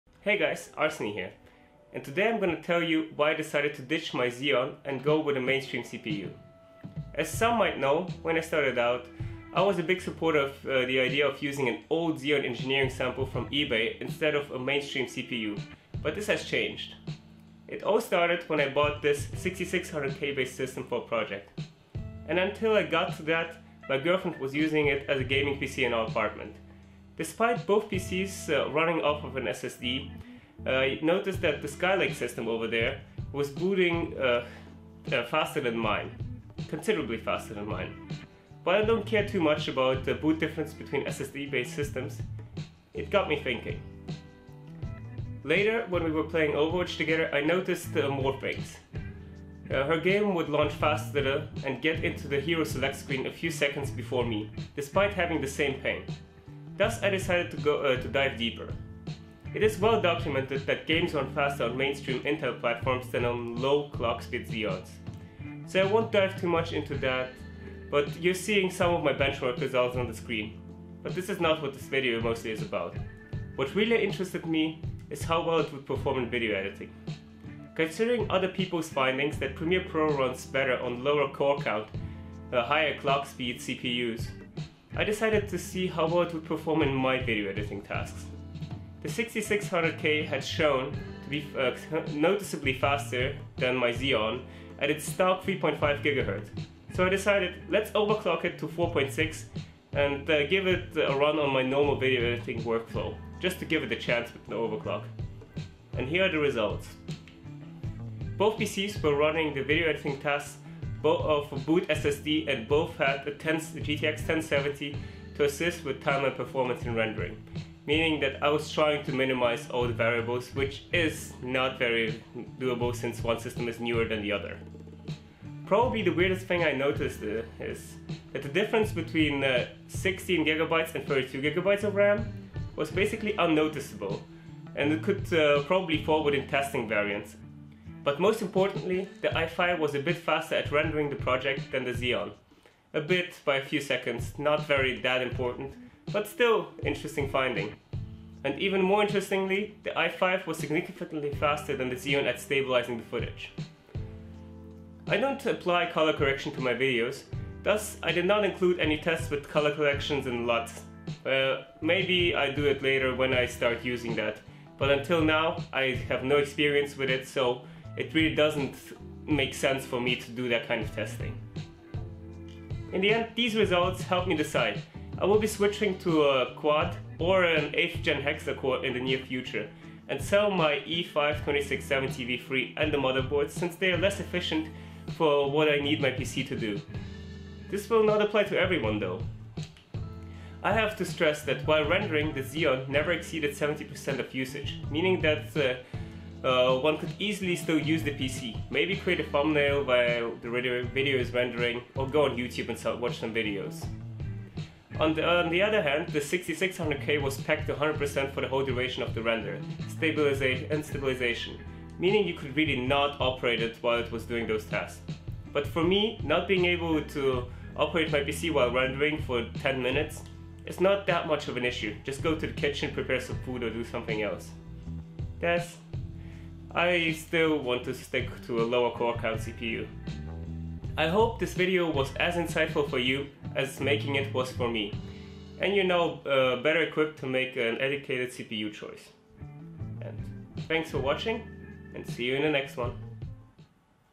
6600K vs e5 2670v3. the video editing showdown, Review
Testing the 6600K overclocked to 4.6Ghz @ 1.275 volts vs my current e5 2670 v3
You might be surprised with the results.